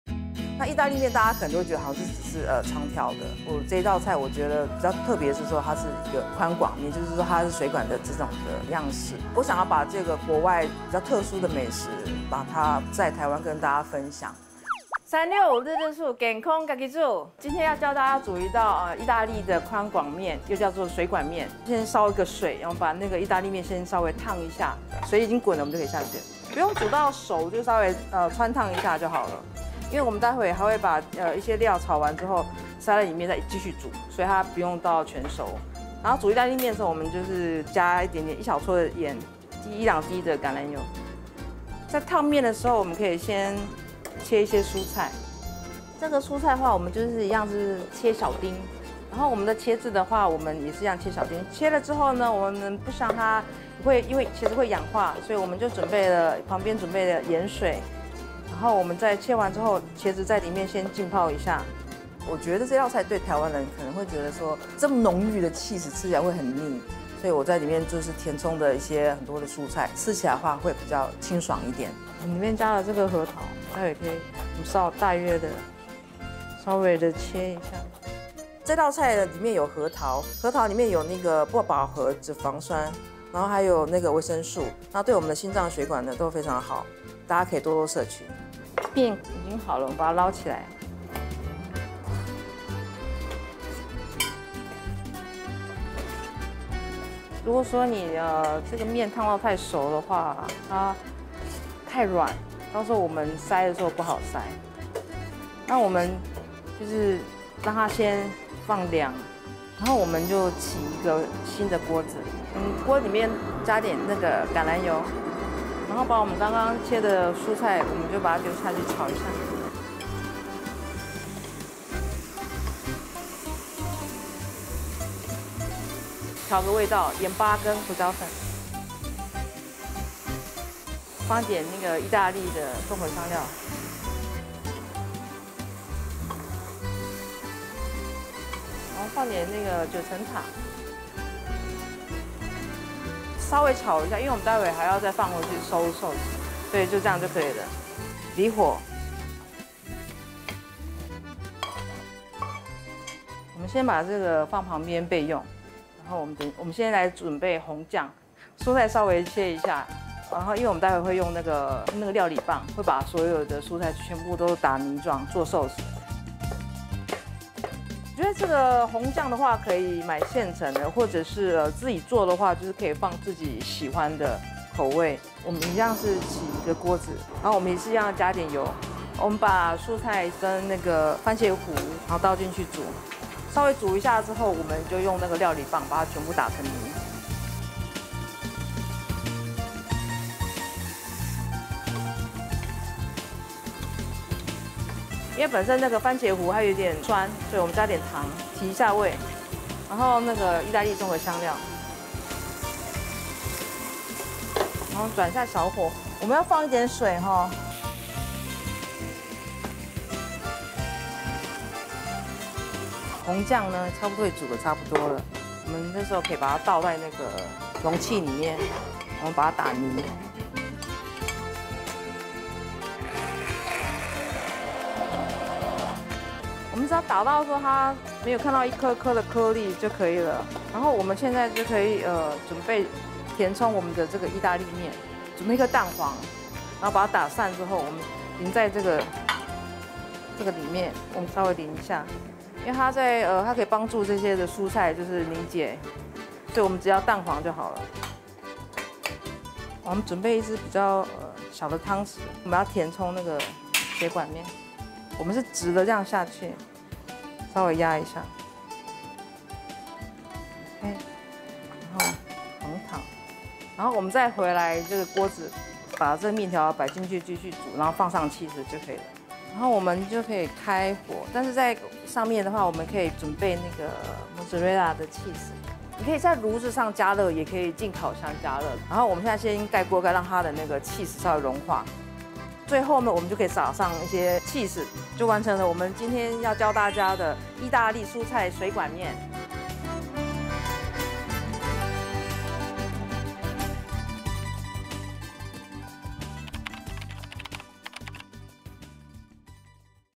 【365日日素 健康自己煮】 義大利蔬菜寬管麵 | 今日主廚：Amy 20240721
義大利麵新吃法！台灣首發，水管麵填滿新鮮蔬菜與核桃，清爽又營養，醬料更是自家熬煮，絕對滿足你的味蕾！快來試試看，一口咬下滿滿義大利風味！

今日主廚﹕Amy( 蔡淑純 )

材料﹕義大利寬管麵、杏鮑菇、西洋芹、茄子、紅蘿蔔、橄欖油、義大利綜合香料、羊奶乾酪(Romano cheese) 、莫札瑞拉起司、核桃、蛋黃、巴西里
紅醬材料﹕番茄糊、紅蘿蔔、紅椒、西洋芹、糖、九層塔、鹽、胡椒